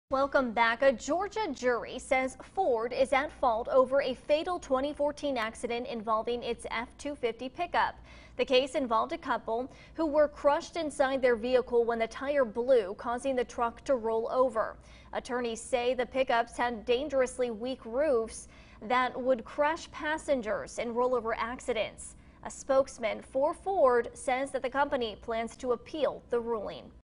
Ford to appeal 1.7 billion verdict in Georgia truck crash
Ford to appeal 1.7 billion verdict in Georgia truck crash. 8/22/22